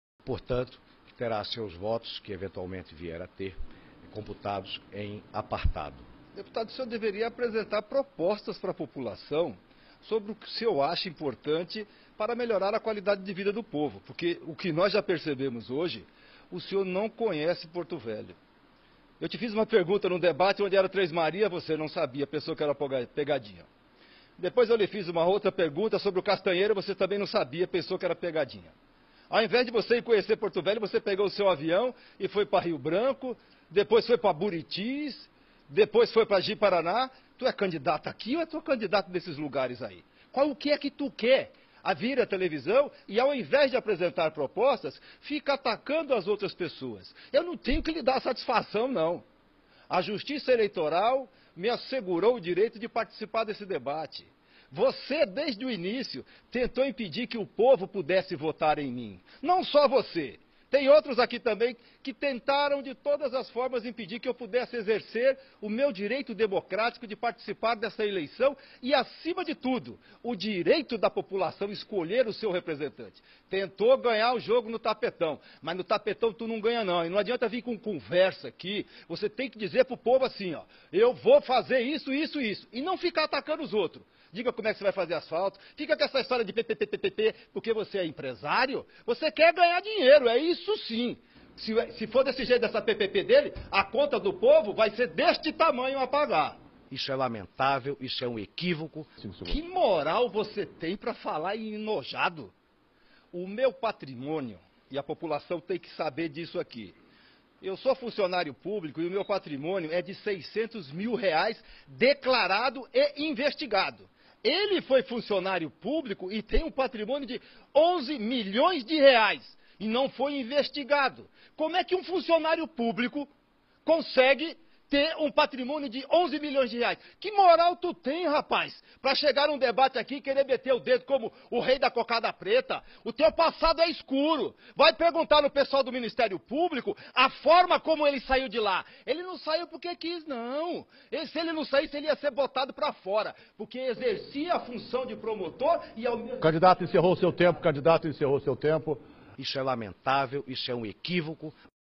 Roberto dá "invertida" em Hildon Chaves
Candidato do PT, todo enrolado na justiça deixa ex-promotor em "saia justa" em debate na TV Rondônia.